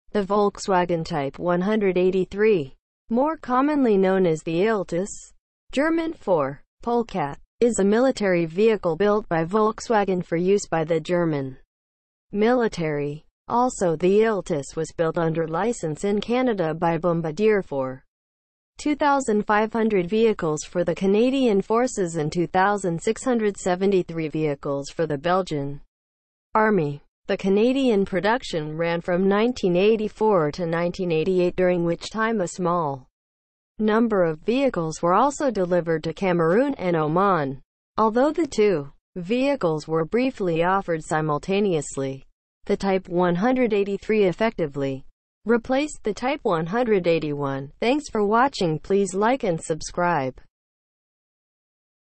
Fast Facts about Volkswagen Iltis
What is Volkswagen Iltis. The meaning of Volkswagen Iltis pronunciation Volkswagen Iltis definition Volkswagen Iltis How to say Volkswagen Iltis

The Volkswagen Type 183 , more commonly known as the Iltis (German for
polecat), is a military vehicle built by Volkswagen for use by the German
military. Also the Iltis was built under licence in Canada by Bombardier for
2,500 vehicles for the Canadian Forces and 2,673 vehicles for the Belgian
Army. The Canadian production ran from 1984 to 1988 during which time a small
number of vehicles were also delivered to Cameroon and Oman. Although the two
vehicles were briefly offered simultaneously, the Type 183 effectively

 BTC: 14TQGMzgJ4uCFv9P5vAHRPk3PxySGVBwrJ